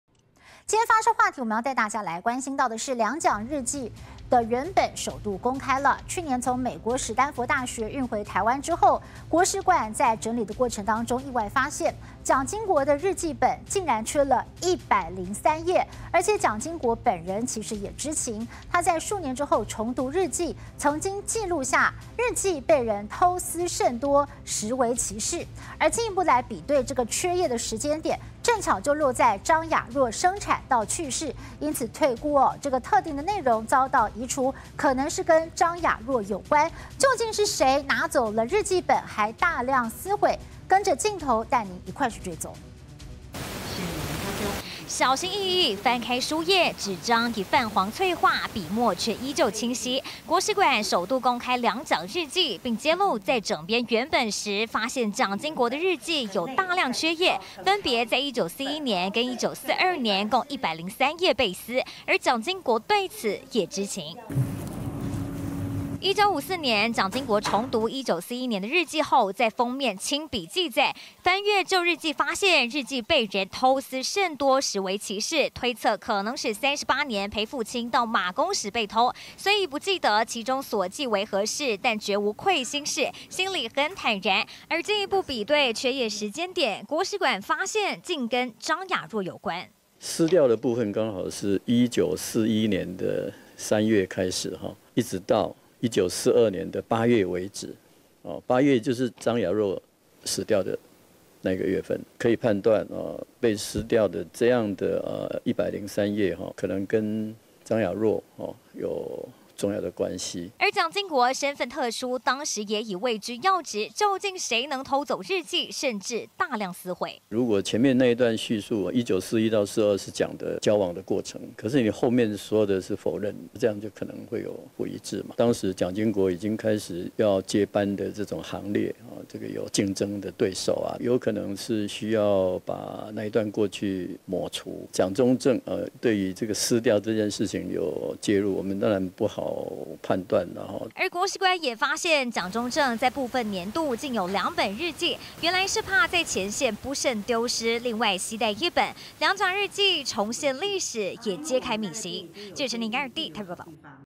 離奇！日記「被偷撕」逾百頁 蔣經國本人也知情
兩蔣「日記原本」 首度公開! 去年從美國史丹佛大學運回後，國史館在整編過程中發現，蔣經國的日記本，竟缺了103頁，而且蔣經國 本人也知情! 他在數年後重讀日記，曾記錄下，「日記被人偷撕甚多、實為奇事」!